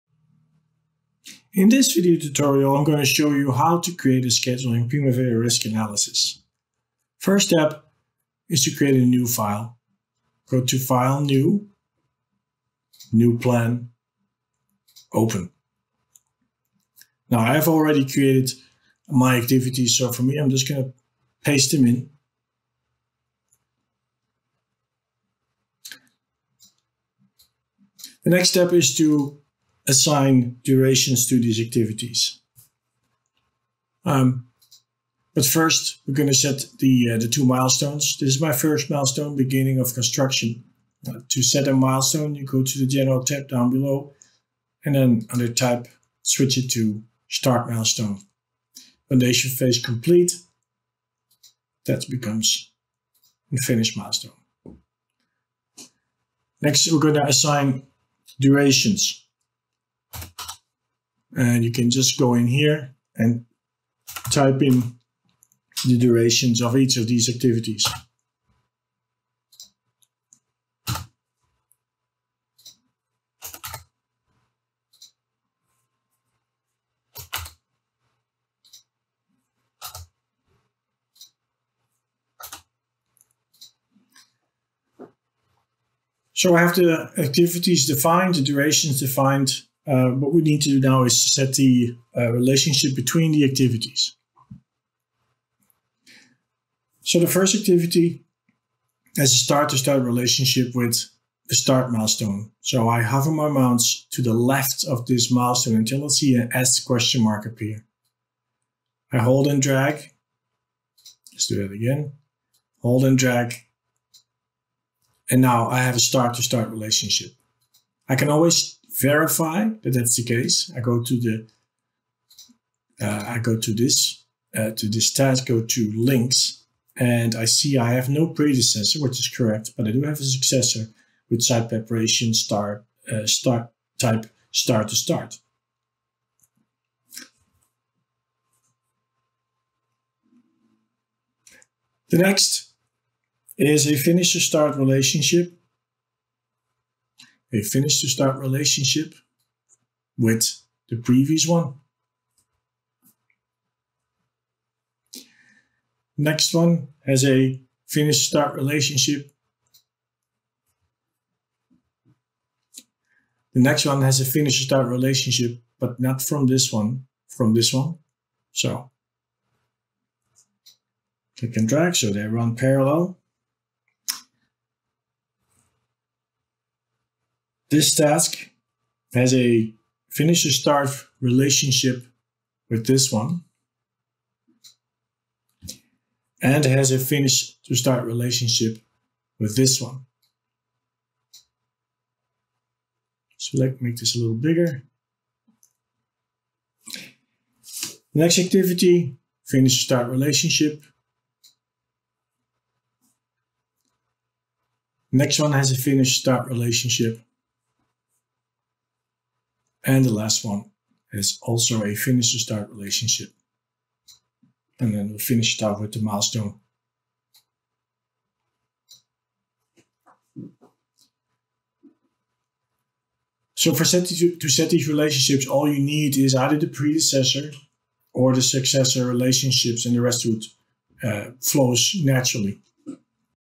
Creating a schedule in Primavera Risk Analysis
This tutorial explains how to create a schedule in Primavera Risk Analysis from scratch.